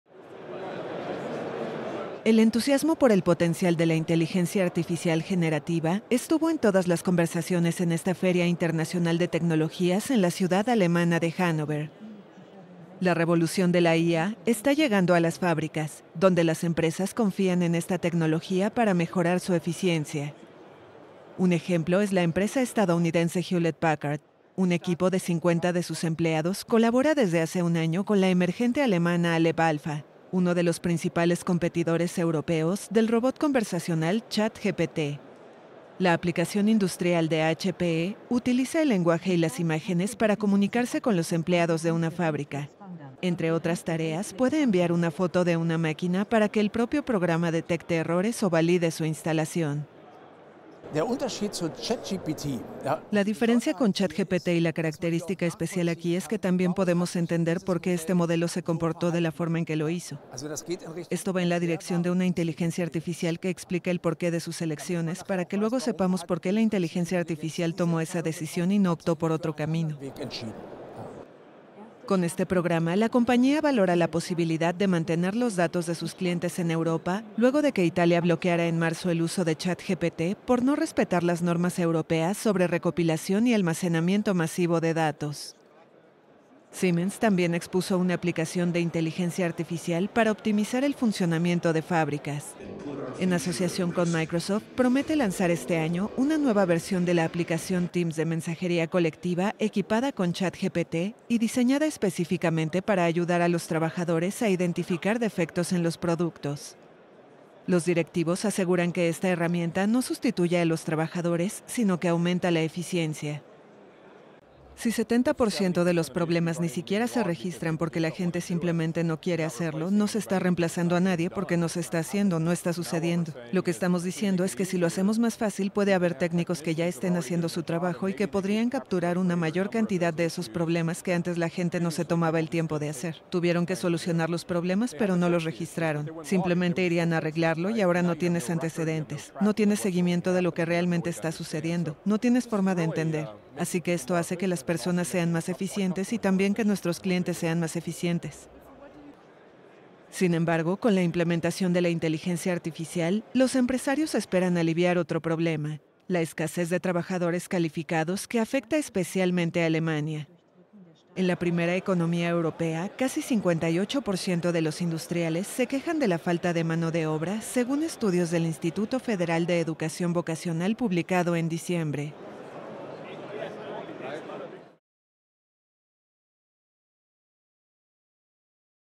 Los empresarios apuestan por usar la inteligencia artificial en las fábricas
La revolución de la inteligencia artificial también está llegando a las fábricas, donde las empresas confían en esta tecnología para mejorar su eficiencia.